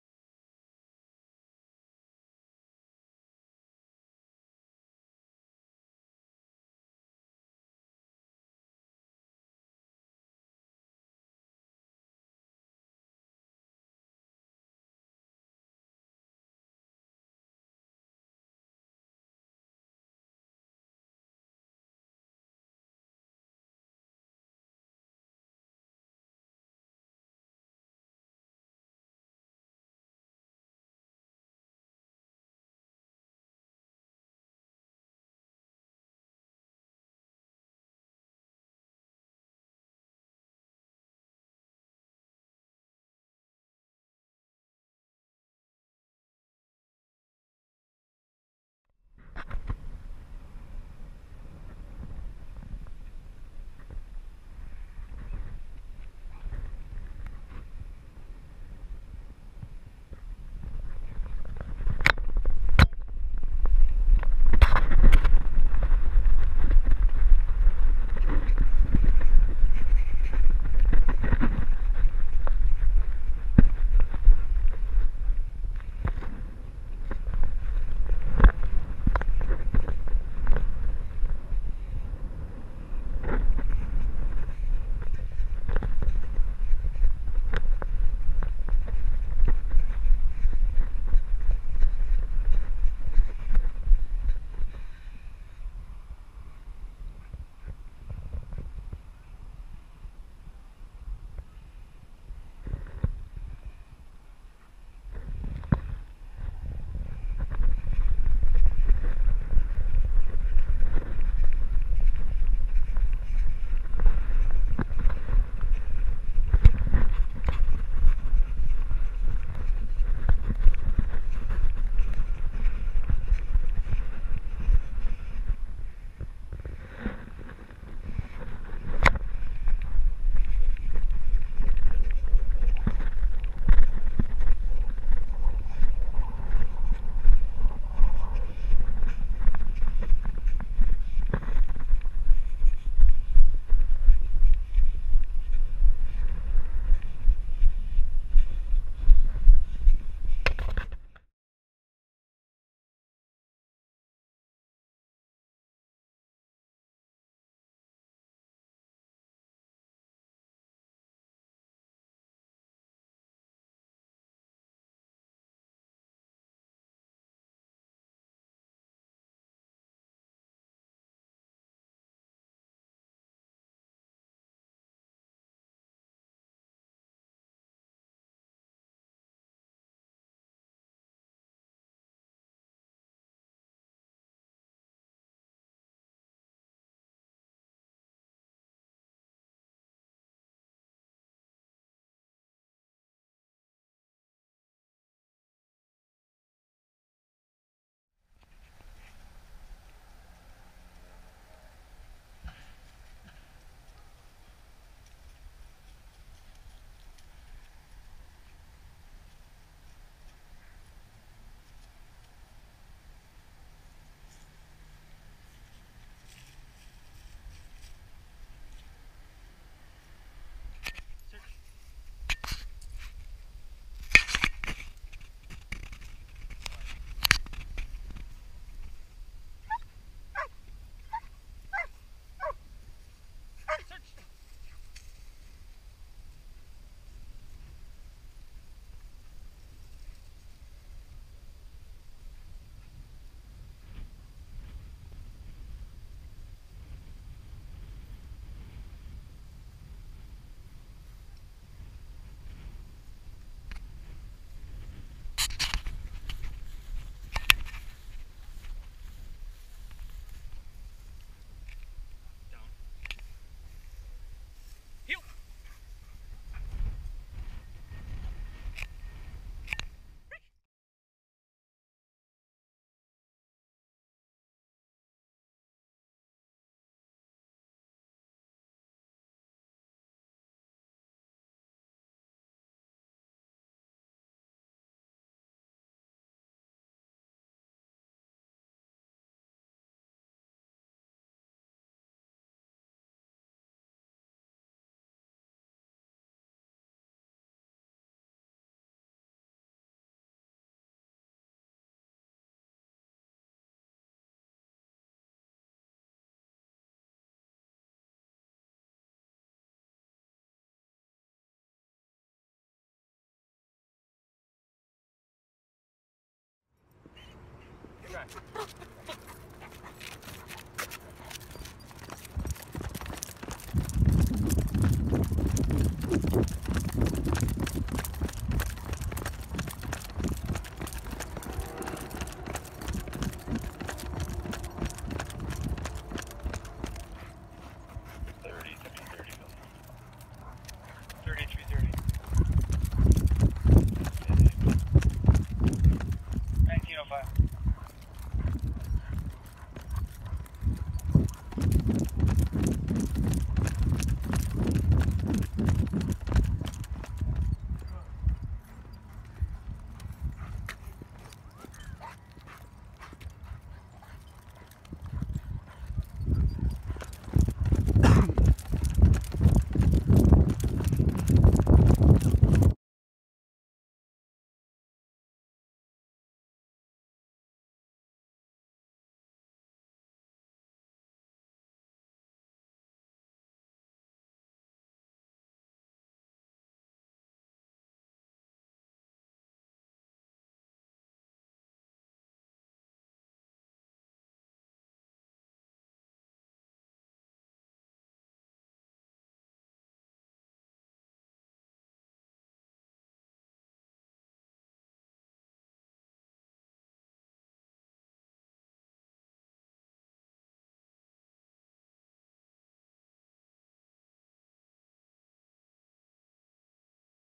South Florida K-9 Inc. photo & video compilation
A compilation of some of the dogs acquired and trained by South Florida K-9 Inc.  A newly formed company located in LaBelle, FL.  Importing GSD, Malinois, and GSD-Malinois cross.  Patrol training, tracking, explosive, and narcotic detection training